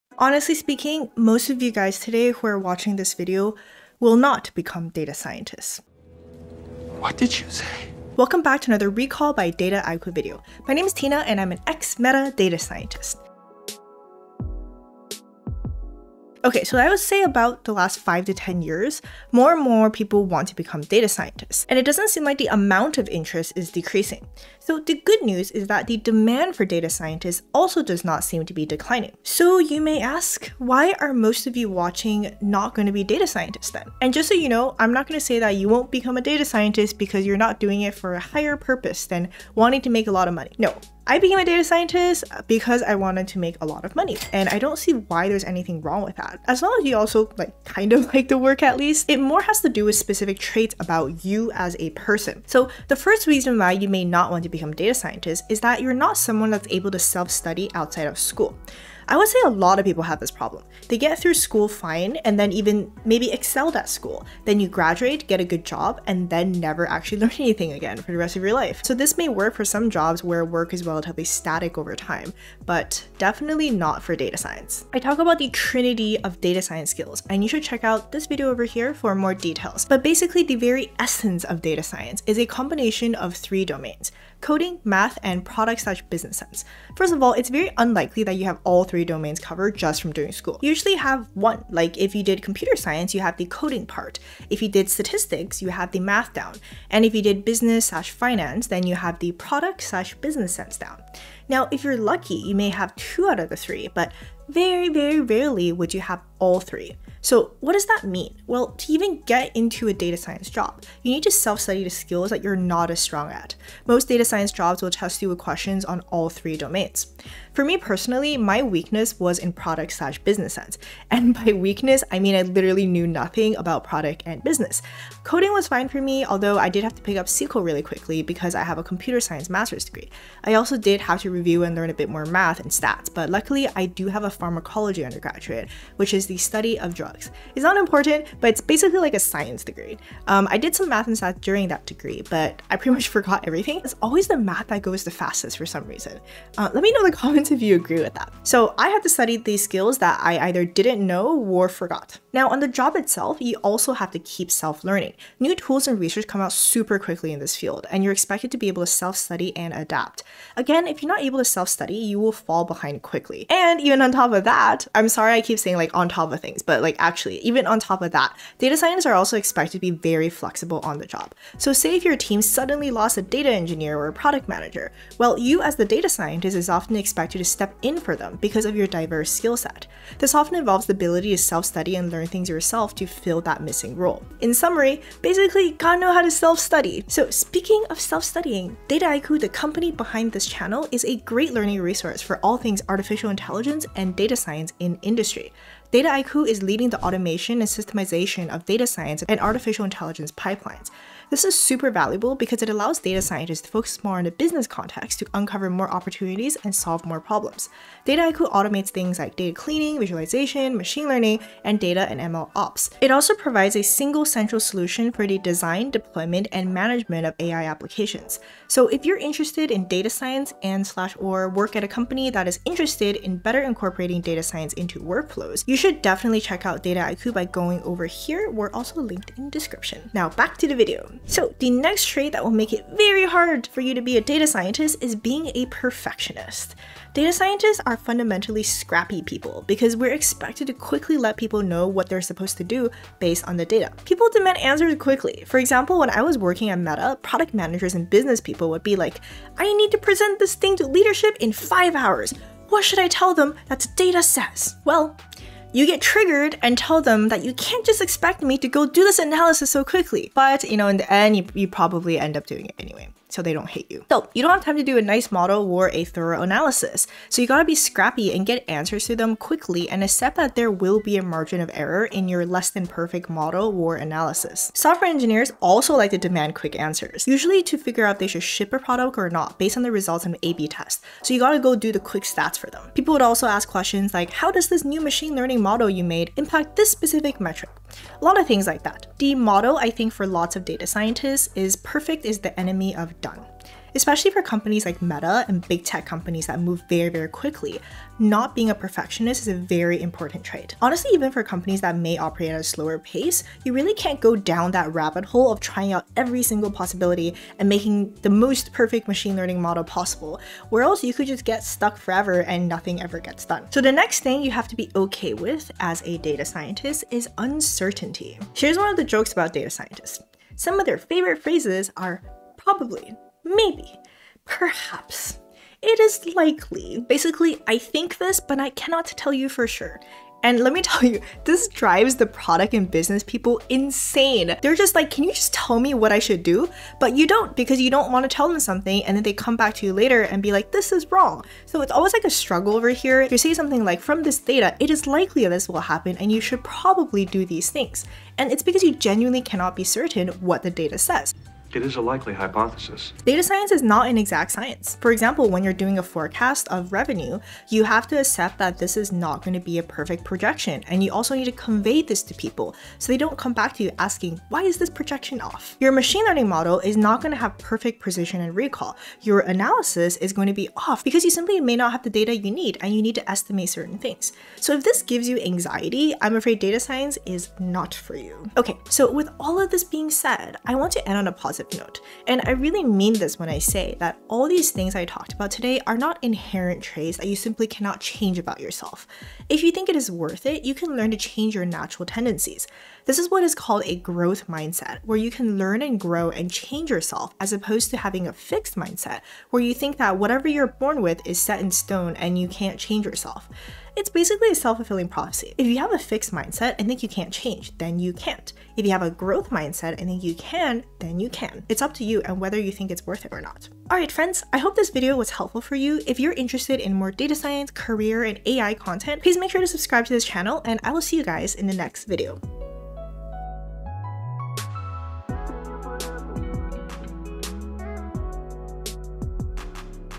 Why You Won't Be a Data Scientist
In this video, Tina talks about the reasons why most of you watching won't become data scientists, which isn't necessarily a bad thing!